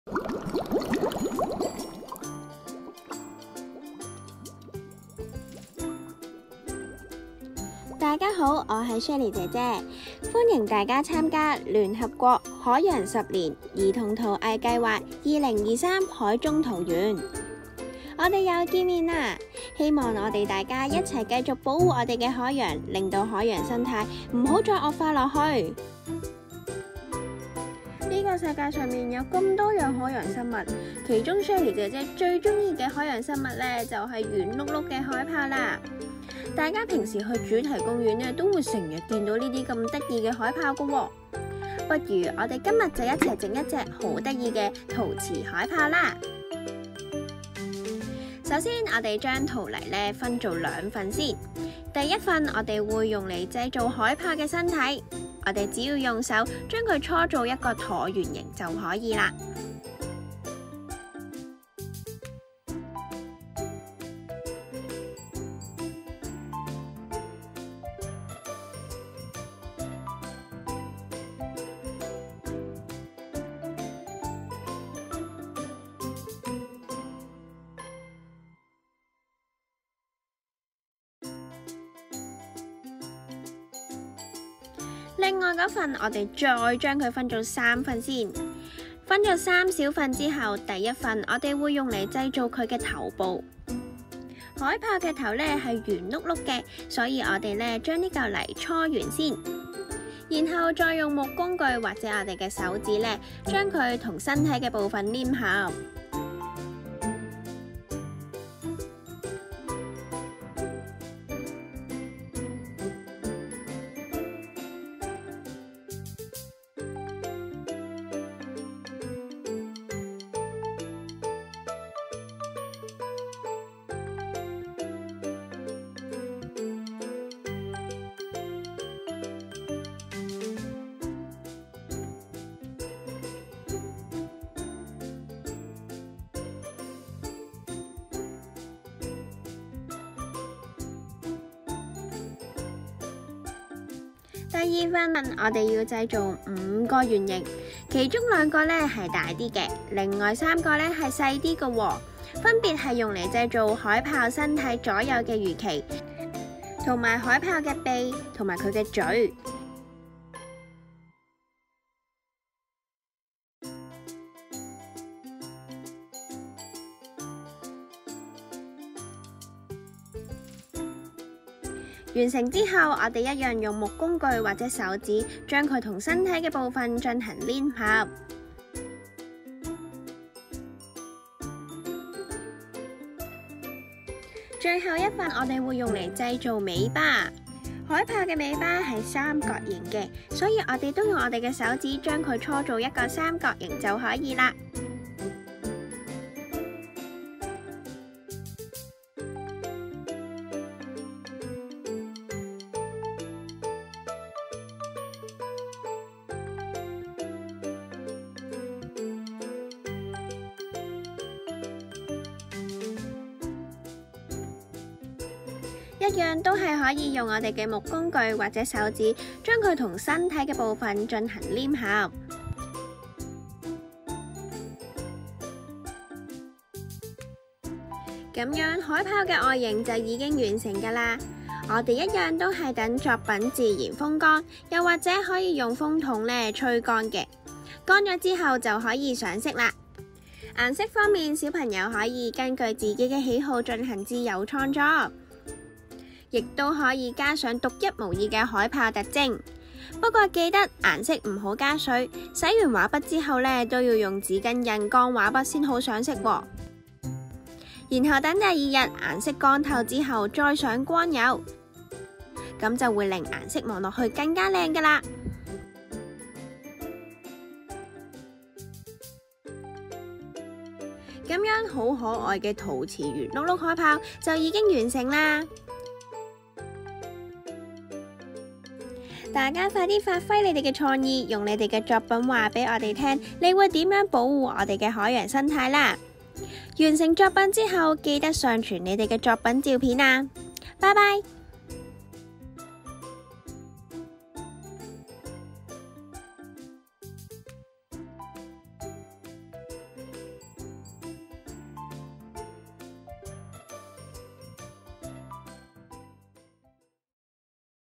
聯合國海洋十年 兒童陶藝計劃 2023「海中陶園」陶藝教學影片 [海豹篇🦭]
2023年聯合國海洋十年的「海中陶園」即將展開，就讓SHIRLEY姐姐👩🏻先在這裡教大家做一隻很可愛的海豹🦭作見面禮☺️🎁。
大家快來發揮創意🖌️，讓我們知道你們會如何愛惜和保護我們的海洋🐠🐟。我們很期待見到大家的作品，快些行動吧！
截止提交作品實物日期🗓️：2023/8/10